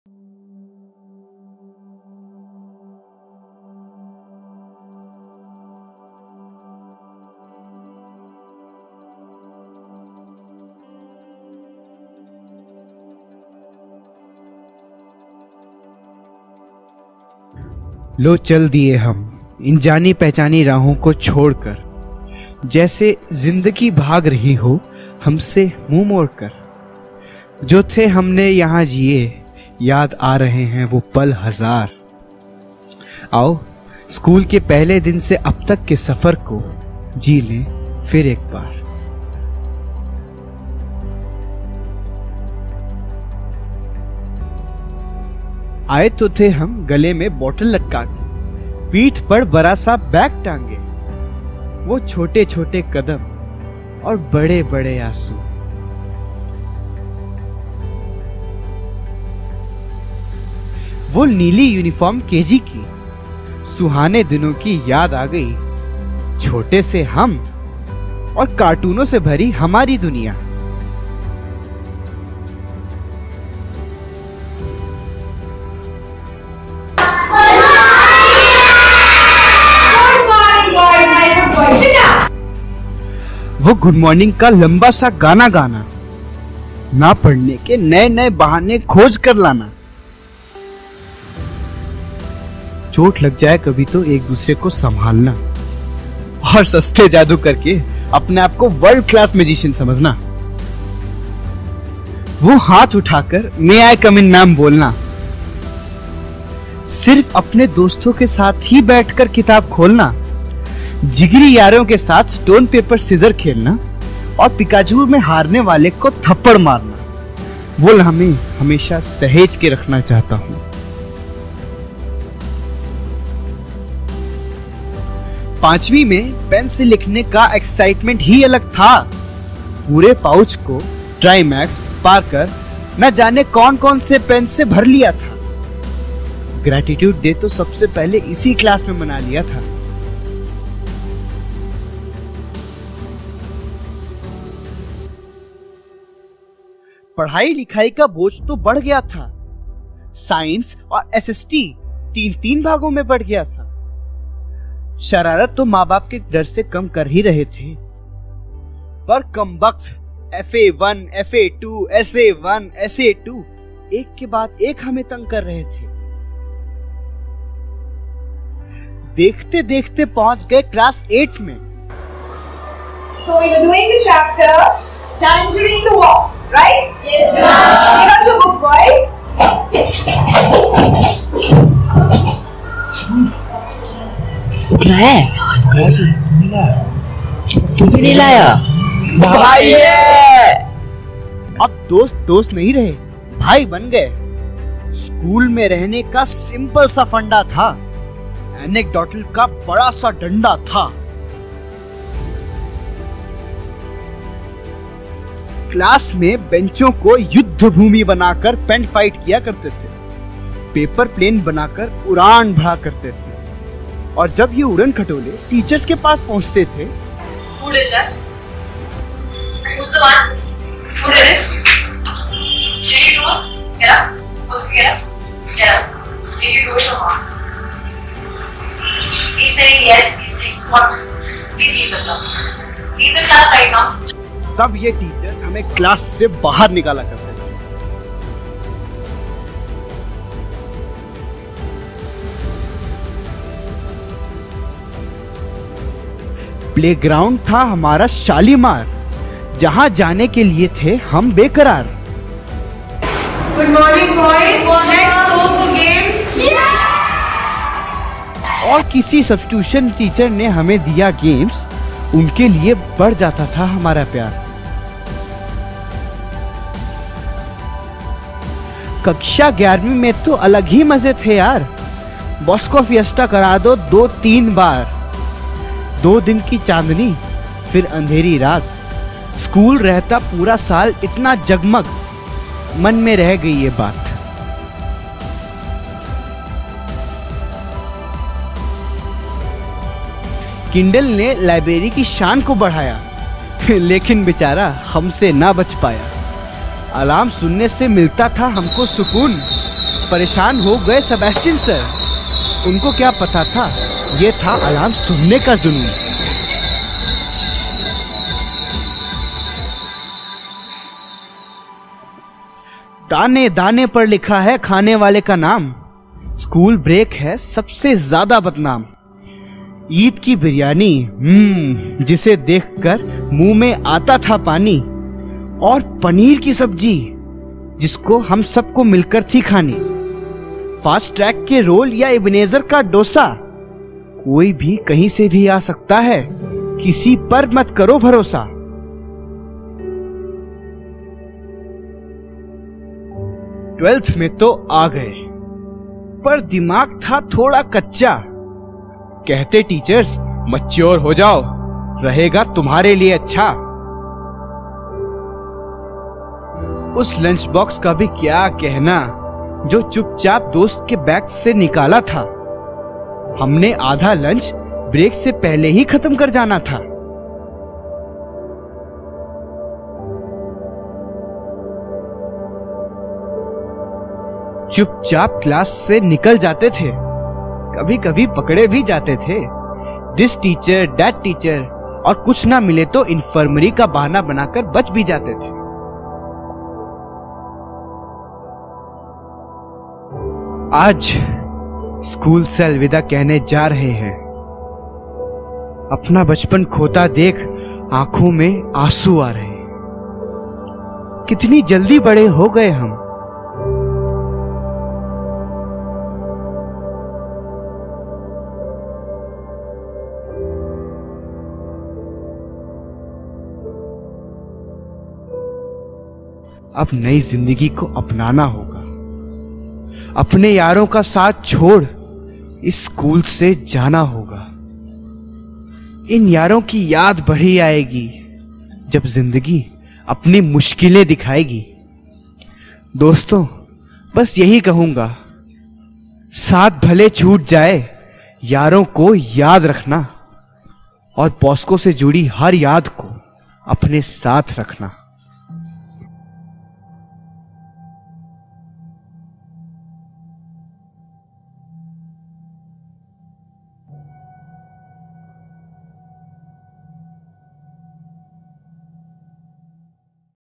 ALVIDA | Class 12th Farewell Video- 2019- 2020 | Don Bosco School, New Delhi
Lo chal diye hum, inn jaani-pehchaani raahon ko chhod kar
Jaise zindagi bhi bhaag rahi ho, humse mooh mod kar
Jo the humne yahan jiye, yaad aa rahe hain vo pal hazaar
Aao, school ke pehle din se abhi tak ke safar ko
Jee lein phir ek baar!!

With Love for the batch of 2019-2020 !
Directed By- Parth Kishan and Jeevan George
Cinematography - Abraham V. George and Anmol
Editing - Parth Kishan and Abraham V. George
Written by - Sarthak Priyadarshi
Voice Over- Harshvardhan Shukla
Creative Inputs - Karuna Ma'am